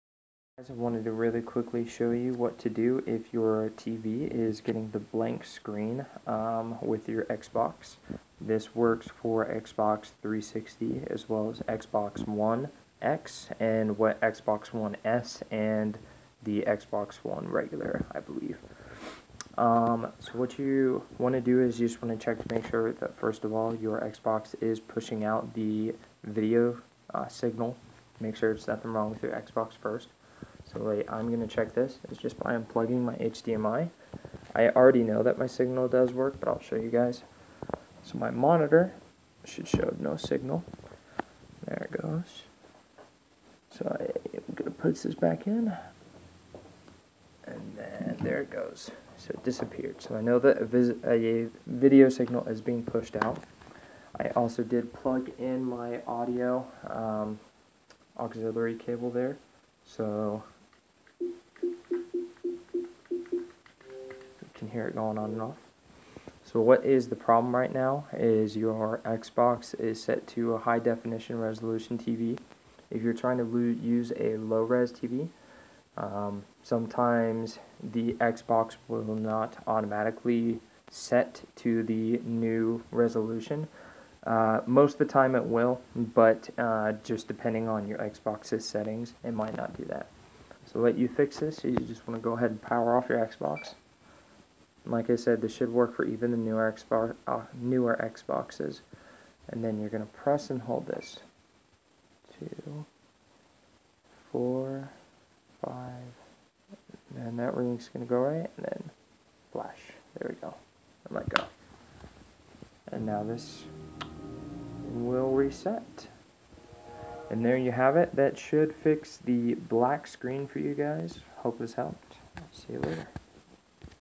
Xbox black screen fix. Works for 360. One. S. X
This is how to fix a black screen connection with any xbox.